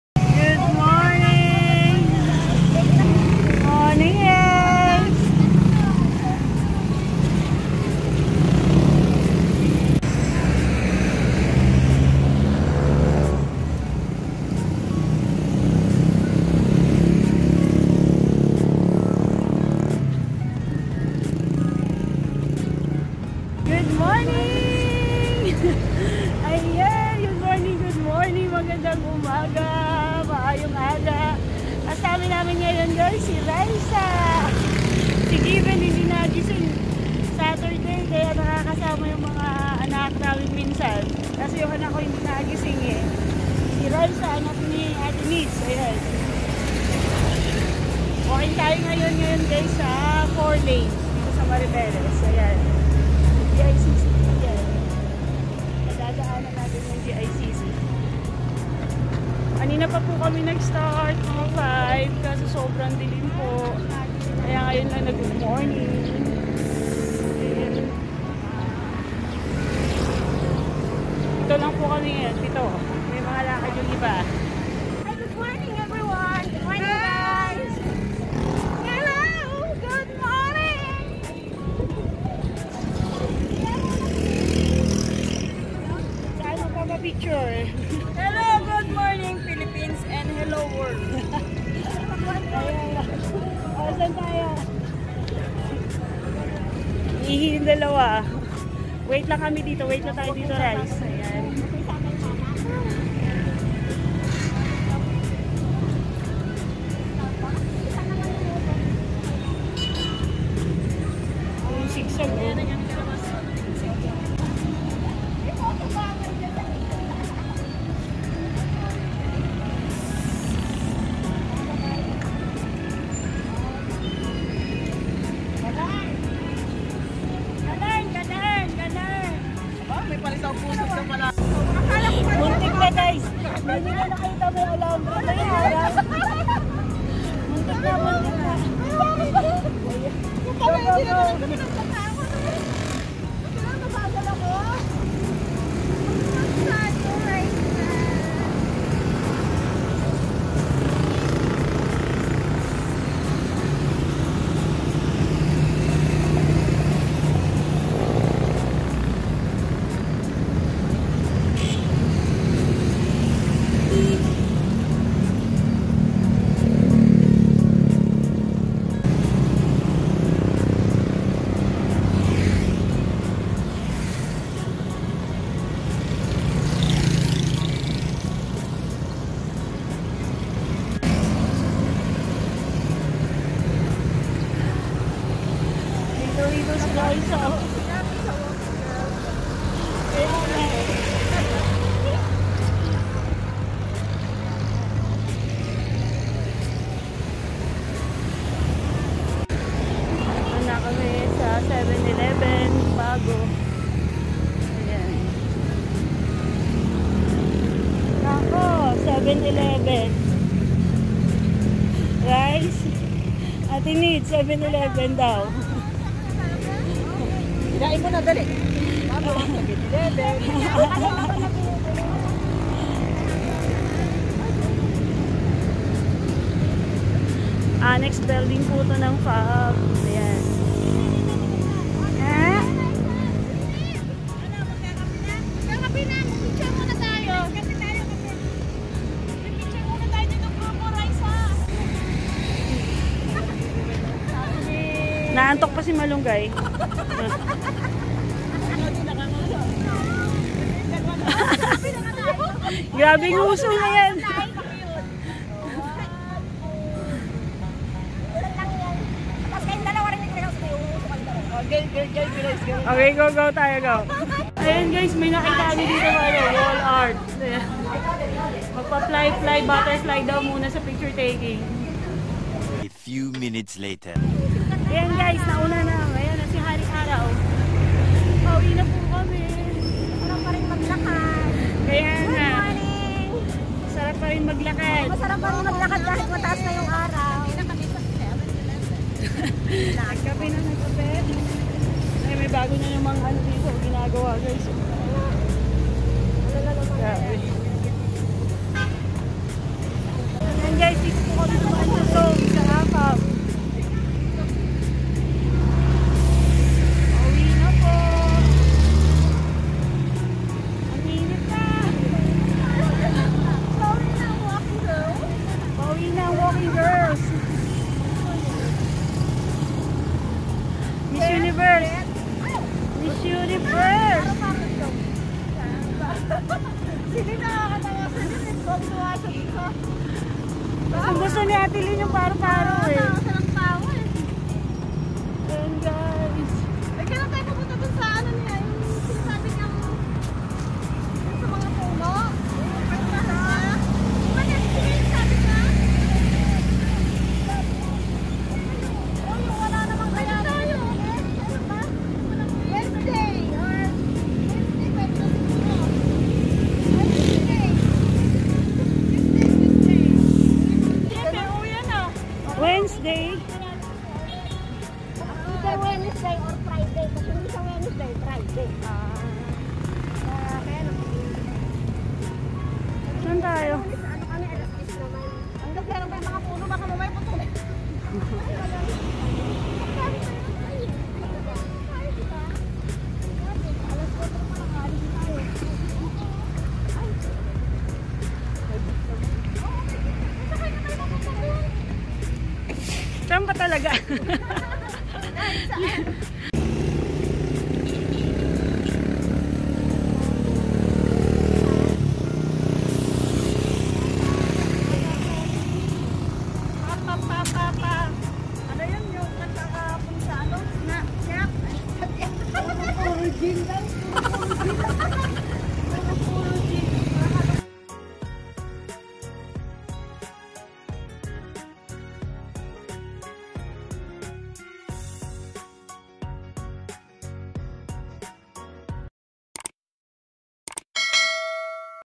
Walkavlog🚶‍♀️#25📍AFAB Mariveles Bataan
Gracian & her Graces| Walking sa Mariveles vlog #25 w/ the Walking Girls Club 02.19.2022

Everybody is getting busy right now, but it's good to be together sometimes and have a walk.

And please also like the facebook page of walking girls club! Thank you!!!